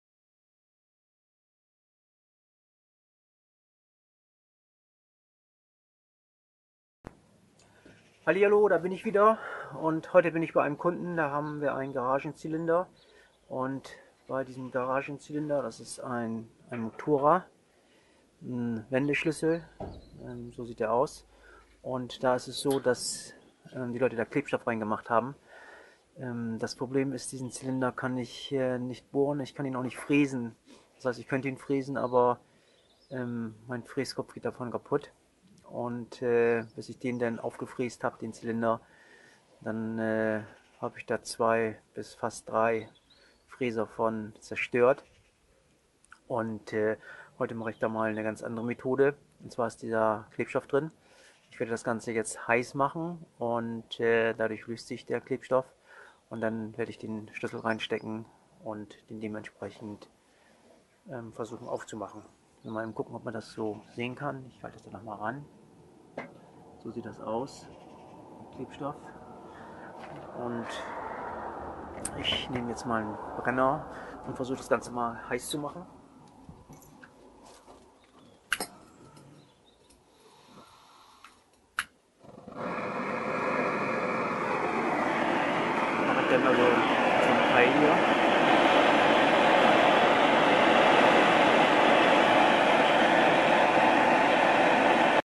Open garage adhesive in the cylinder Locksmiths Oldorf seit 1906
The cylinder is an adhesive the cylinder is a Mottura.
Not to Drill

Schlosserei & Schlüsseldienst seit 1906
Am Lehester Deich 64 c
28357 Bremen